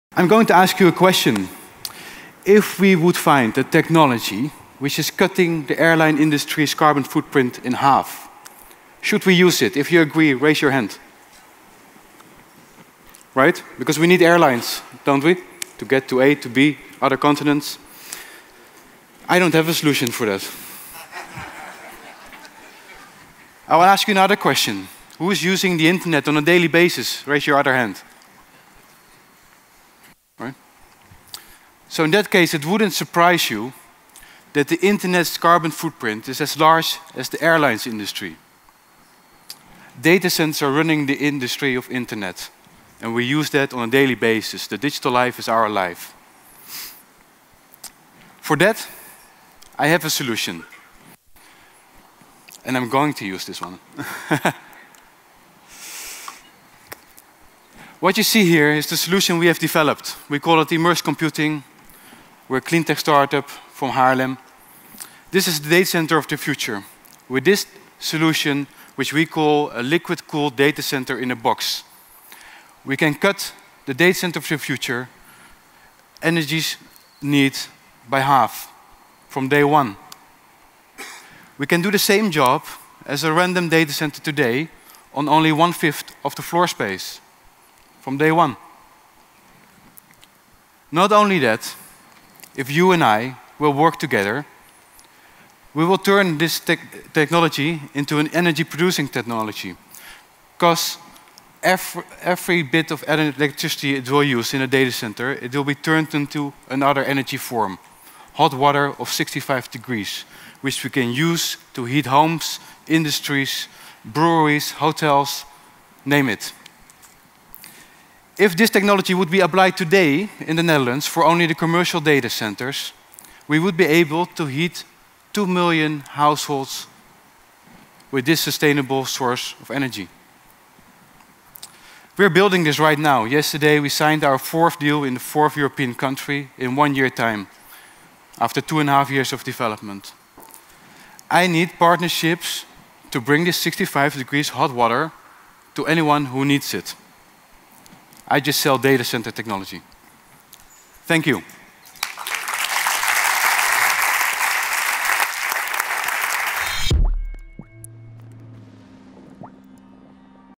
Asperitas Pitch New Energy Challenge 2018
Watch Maikel Bouricius’s award winning pitch at the Shell New Energy Challenge